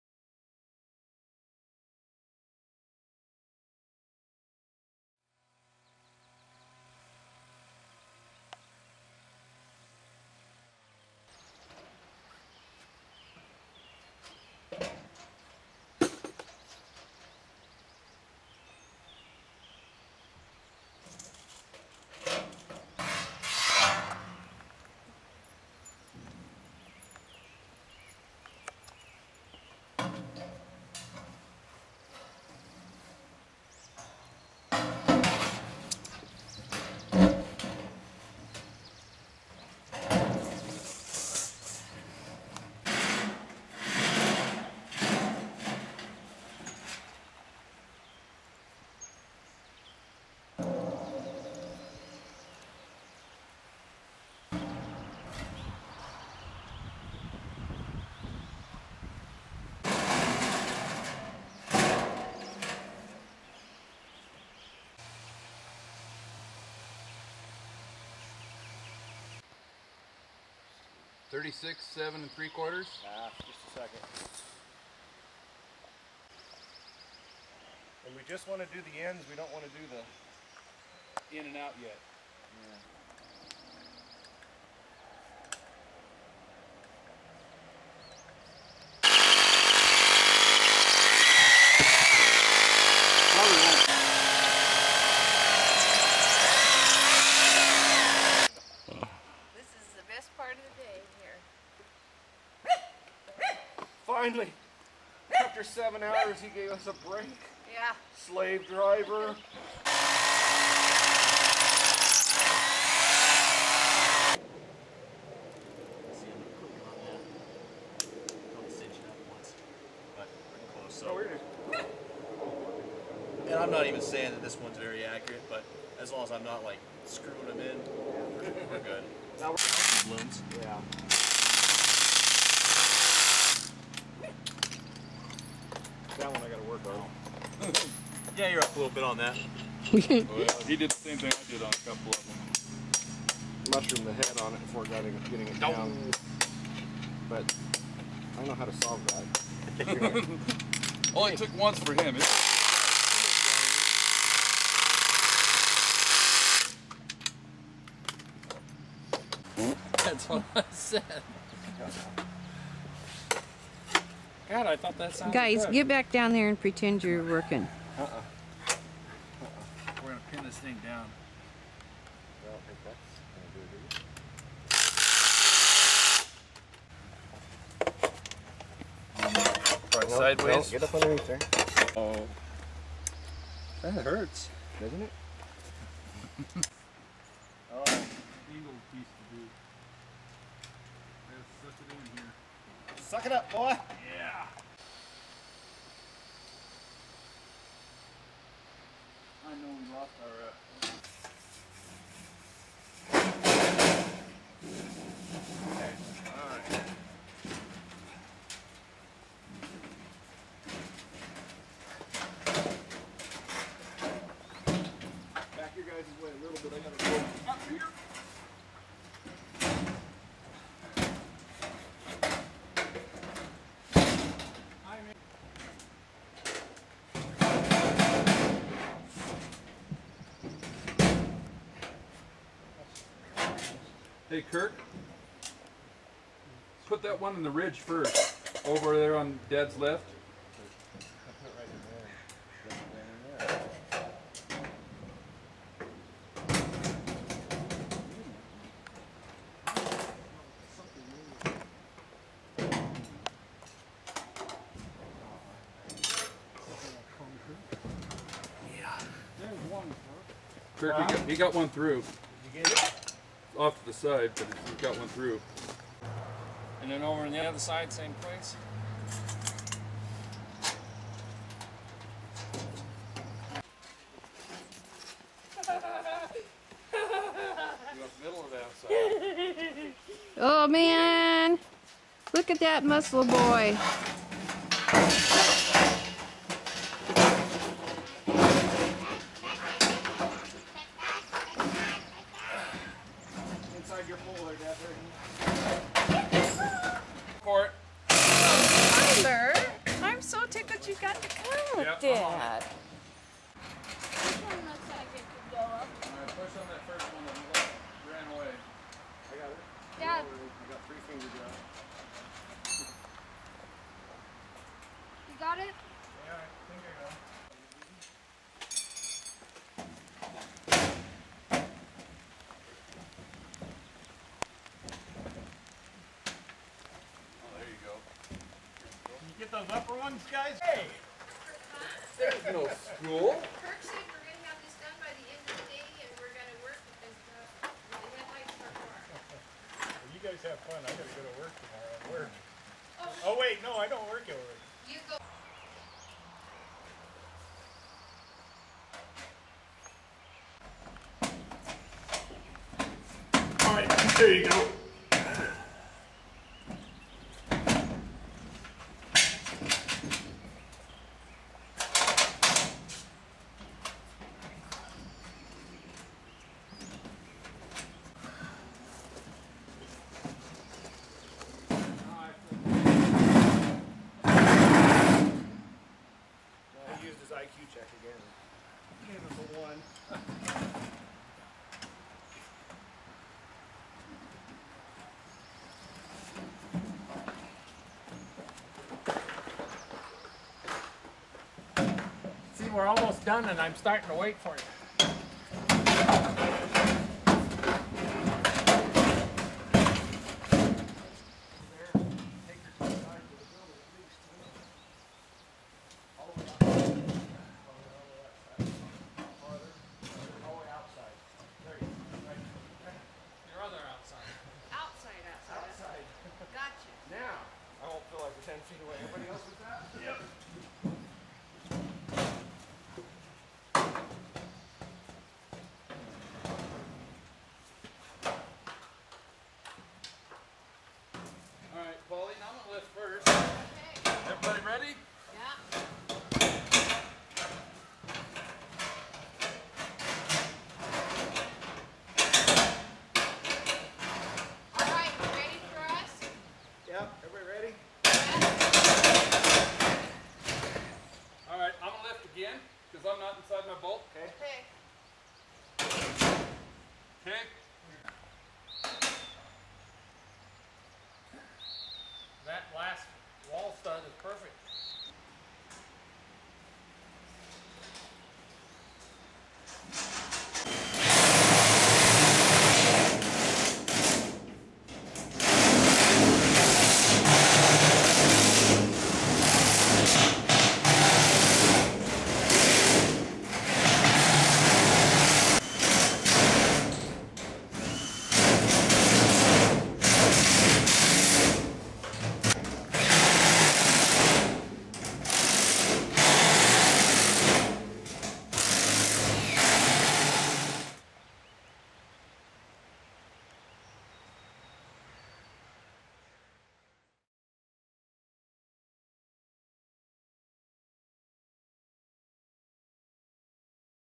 Steel Building Construction
An Oregon family builds a SteelMaster quonset hut in just a few days.  This video shows the building construction step-by-step. The building is being used as a steel boat barn.

Ready to start your own?